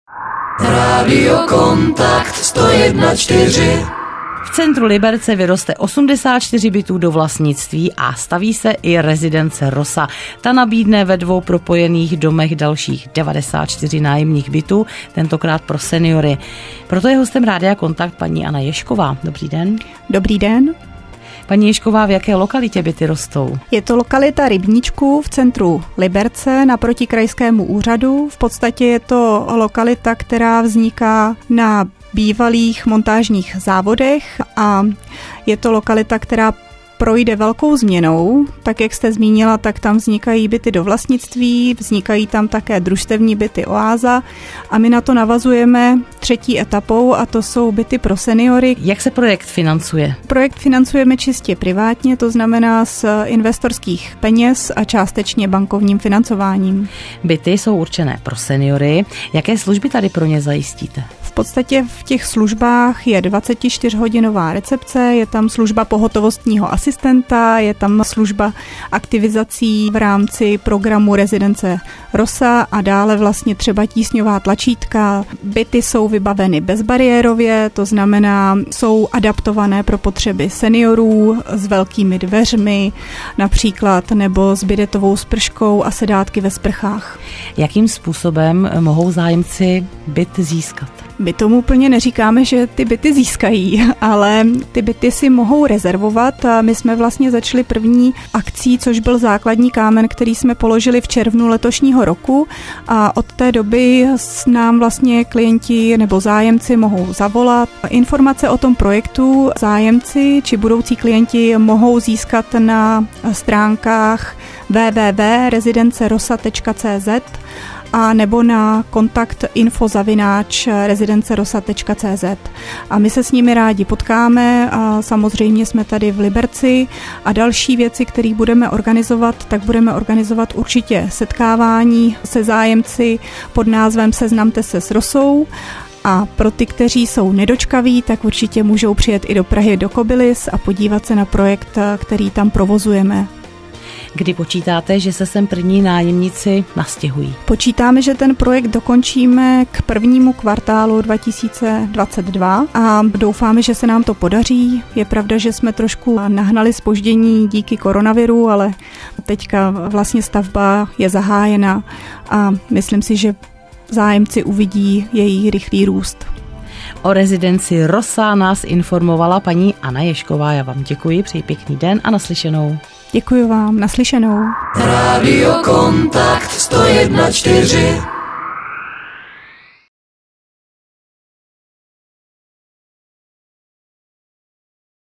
Rezidence RoSa Liberec - rozhovor Anna Ježková
Rozhovor Anny Ježkové o vznikající Rezidenci RoSa v Liberci na radiu Contact Liberec